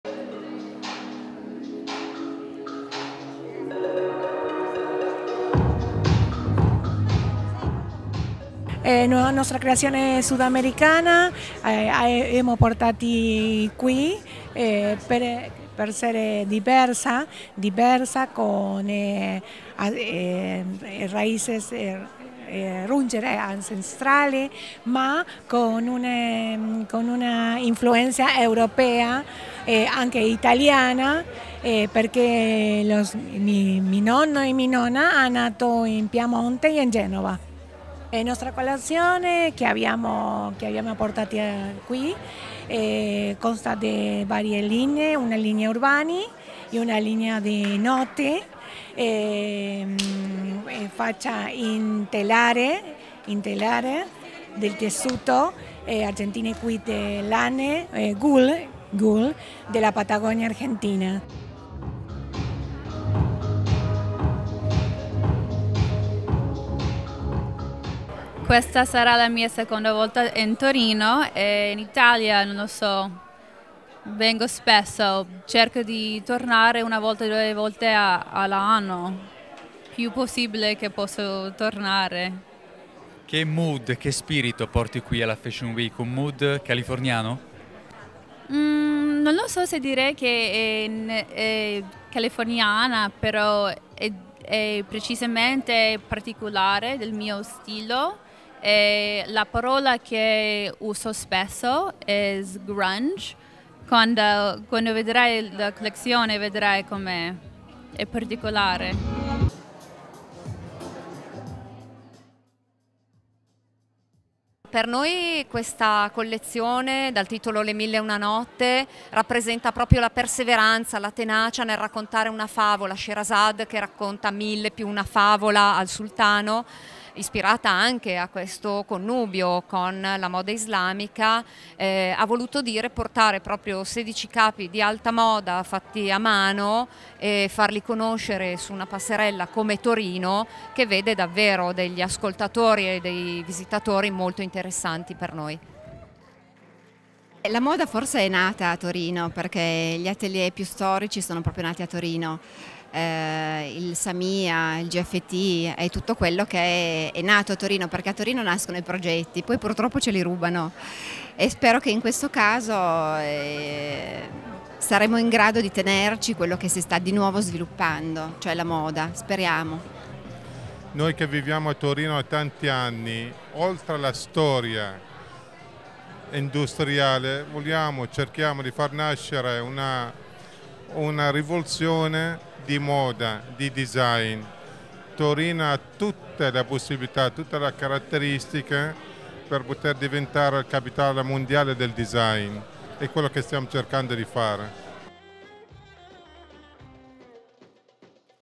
Torino Fashion Week 2017, gli stilisti che sfileranno
(Agenzia Vista) - Torino, 29 Gugno 2017 - Seconda serata di passerella per gli stilisti presenti alla seconda edizione di Torino Fashion Week, la settimana della moda piemontese. Tanti gli eventi nel corso della settimana, tutti legati alla moda a 360 gradi, nella location dei Murazzi del Po, ai magazzini Devalle. La settimana della moda a Torino, organizzata dall’associazione TMODA con il patrocinio della Città di Torino, prosegue fino al 3 Luglio. Courtesy Rete 7